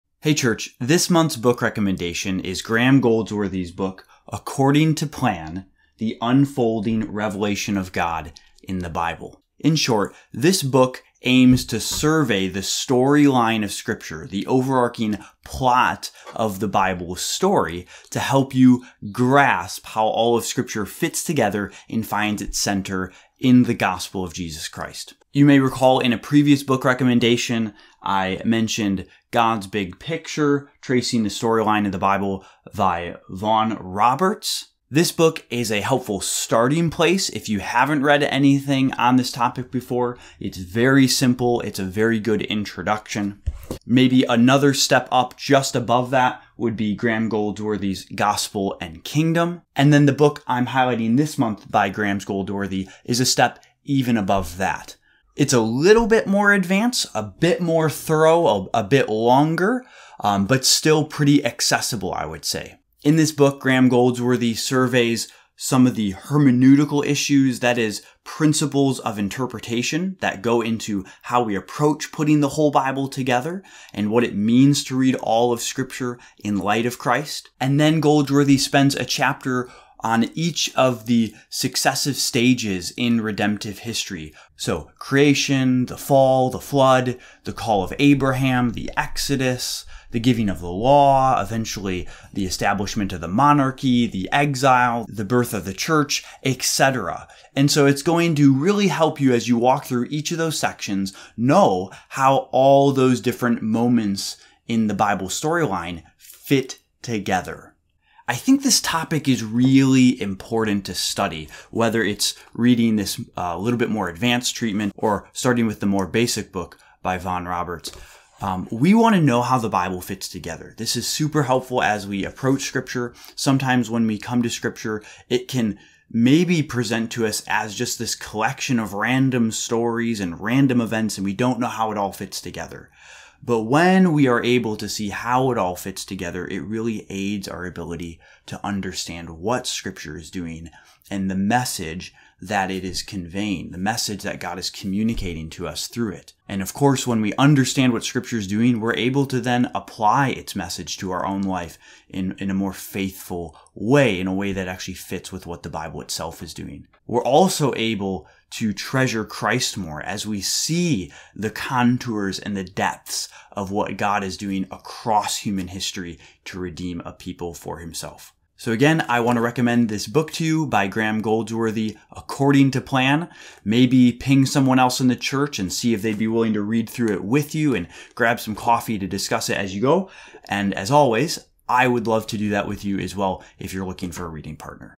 According to Plan by Graeme Goldsworthy (Book Recommendation)
How does the whole Bible, in all its various parts, fit together to tell the story of God's redemptive plan accomplished by Christ? This month's book recommendation is According to Plan: The Unfolding Revelation of God in the Bible by Graeme Goldsworthy.